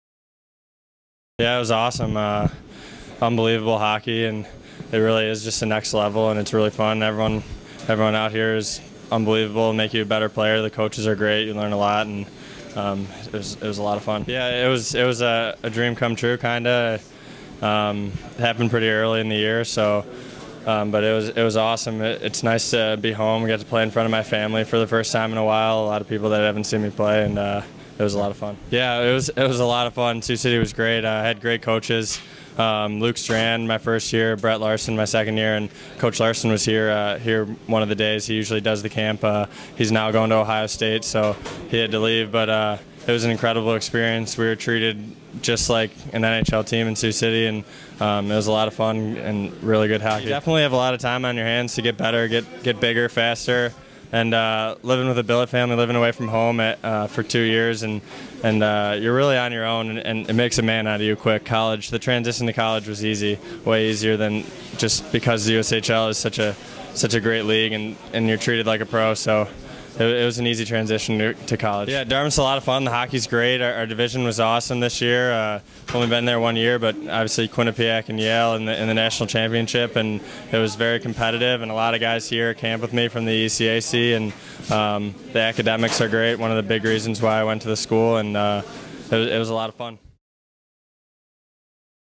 USHL Alumni Voices: Tim O'Brien
Sioux City Musketeers alum Tim O'Brien reflects on his time in the USHL and and how it prepared him for college. He also talks about his time at Chicago Blackhawks development camp.